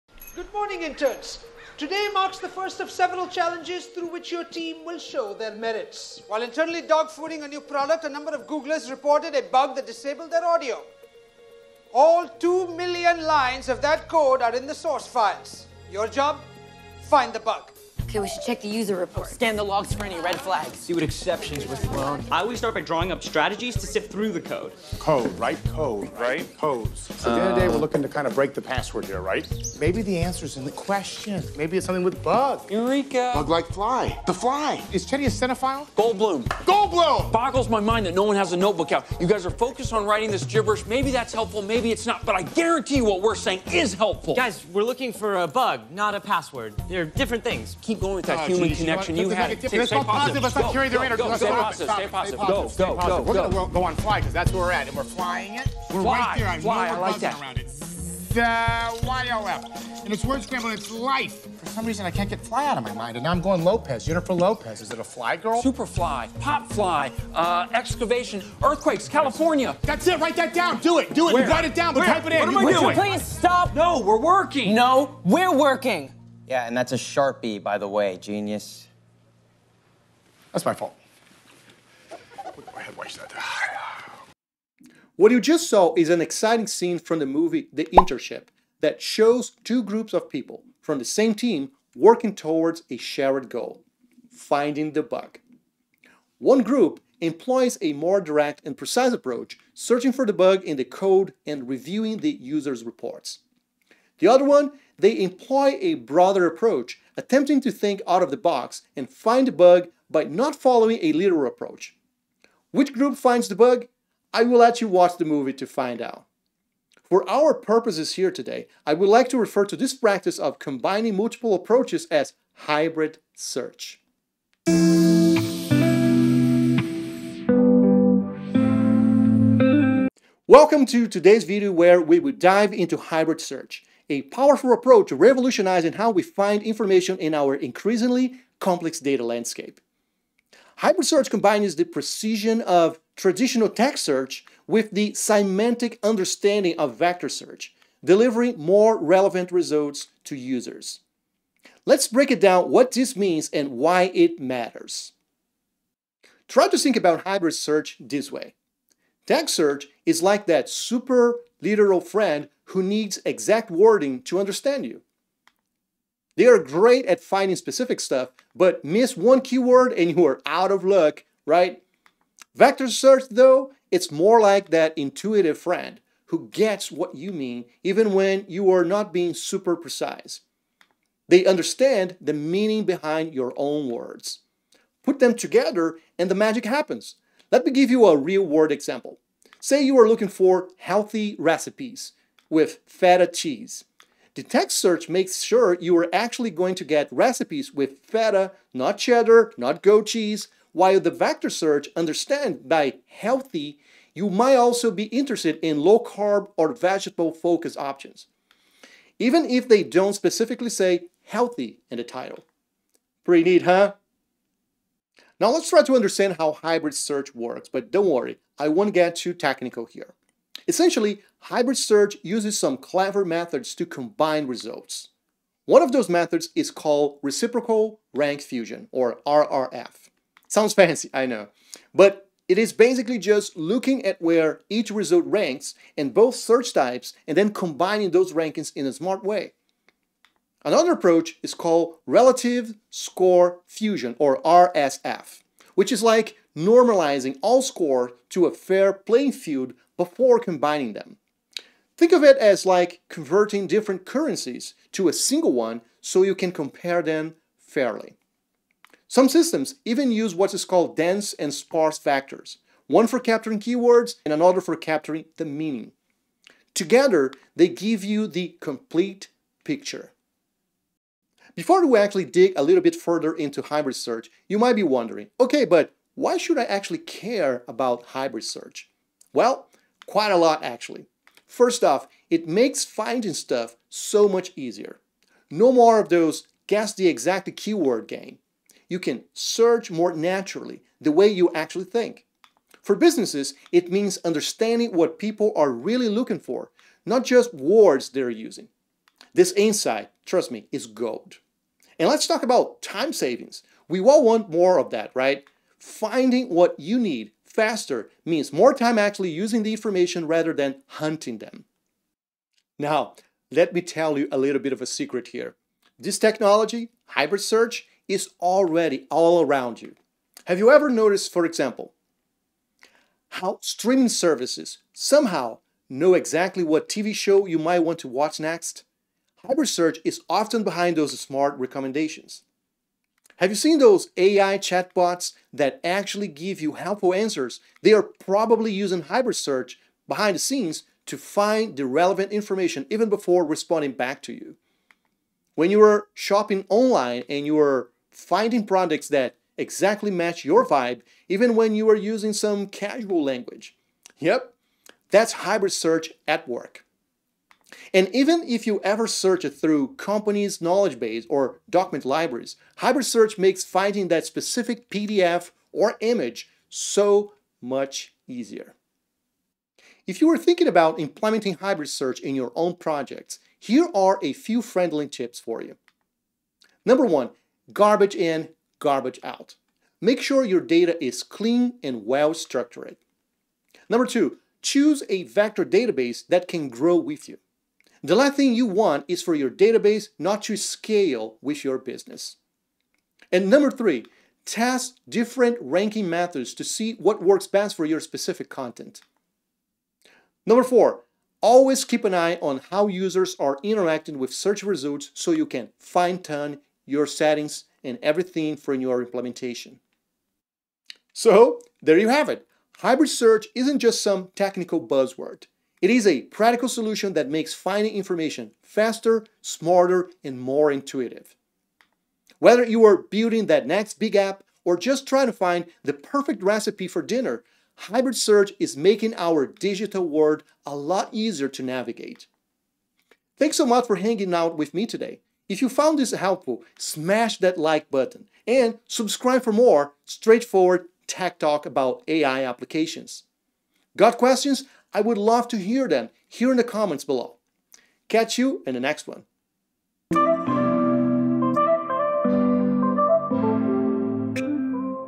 What is Hybrid Search?
Are you building search applications that need both precision AND semantic understanding? Hybrid search might be the solution you're looking for. In this comprehensive video, we explore how combining text search with vector search delivers the most relevant results to your users.

You'll learn:
✅ What hybrid search is and why it outperforms traditional search methods 🔍
✅ Why you should consider implementing hybrid search in your applications ⁉️
✅ Practical techniques like Reciprocal Rank Fusion (RRF) and Relative Score Fusion (RSF) 📊
✅ Real-world applications from recommendation engines to AI chatbots 🤖
✅ Implementation considerations to ensure your hybrid search system delivers results 🚀

Whether you're enhancing e-commerce search, building content discovery platforms, or developing retrieval-augmented generation for LLMs, hybrid search combines the best of both worlds. Get ready to dramatically improve search relevance and user experience in your applications. Ready to supercharge your search capabilities? Let's dive in!

0:00 - Intro
2:10 - What is hybrid search?
3:35 - How hybrid search works?
4:31 - Why should you care about hybrid search?
5:12 - Is hybrid search a reality today?
6:09 - Implementation considerations
6:50 - Summary